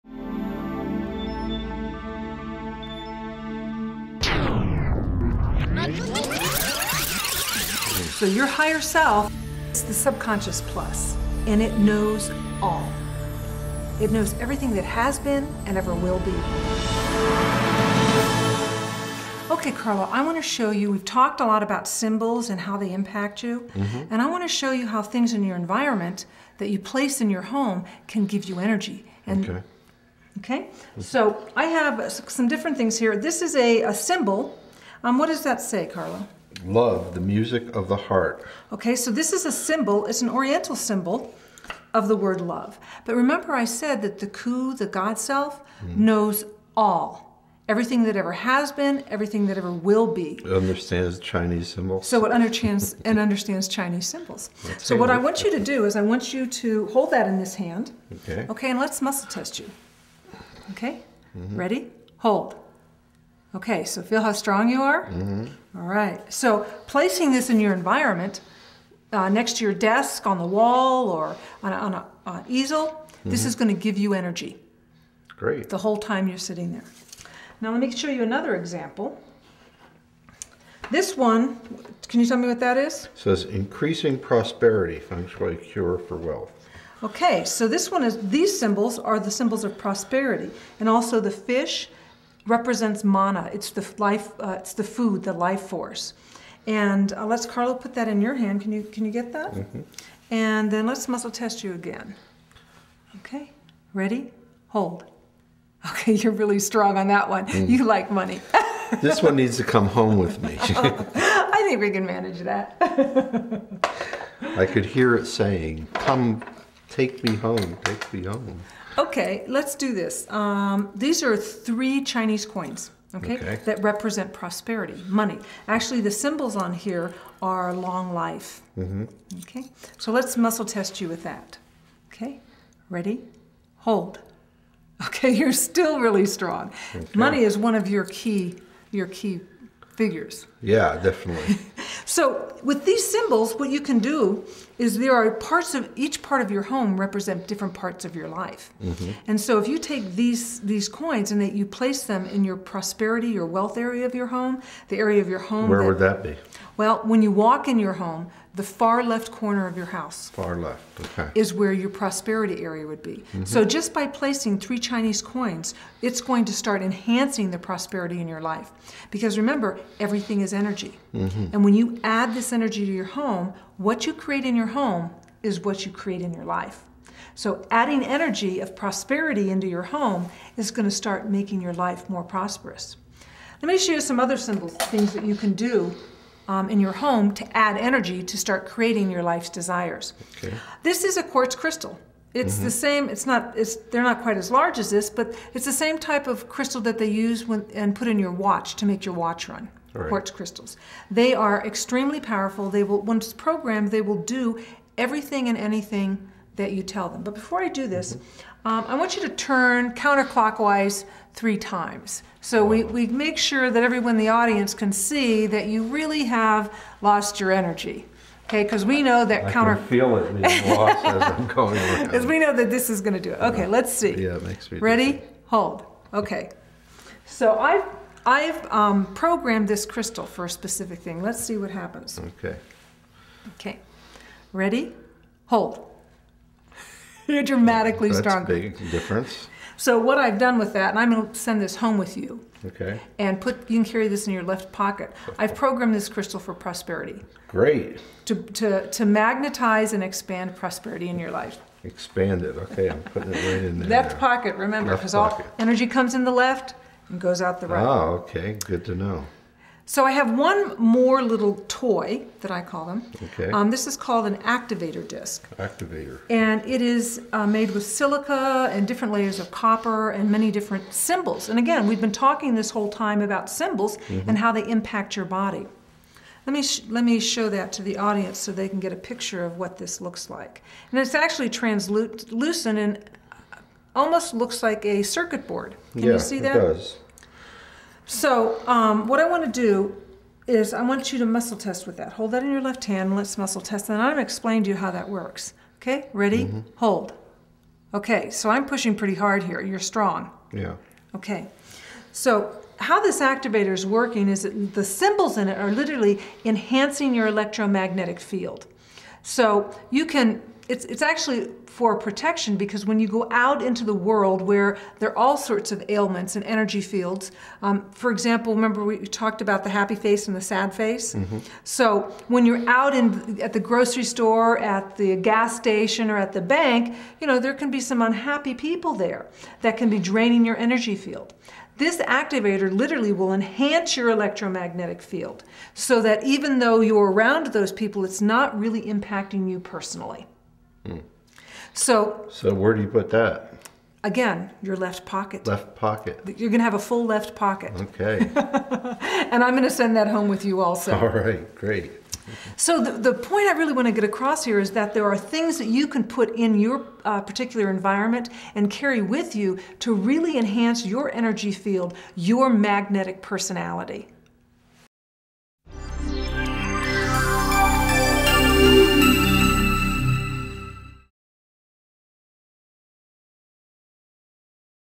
The Secret to Creating a Magnetic Personality
Your environment plays a very important role in helping you create your electromagnetic field.  There are specific items you can place in your surrounding and carry with you to enhance and enlarge your energy field and help you in creating a magnetic personality. The law of attraction includes the items you choose to keep in your environment.  People like Oprah, Madonna and Donald Trump have used this knowledge in their own success. The secret the authors of The Secret, John Gray, Jack Canfield, Bill Harris, Bob Proctor, John Assarf , Abraham and Esther Hicks are not telling you about. This self help, self improvement, self empowerment and personal development video will open your eyes to new ideas. Abundance and Prosperity, love and relationships and success can be yours.  How to get the the Law of Attraction working for you.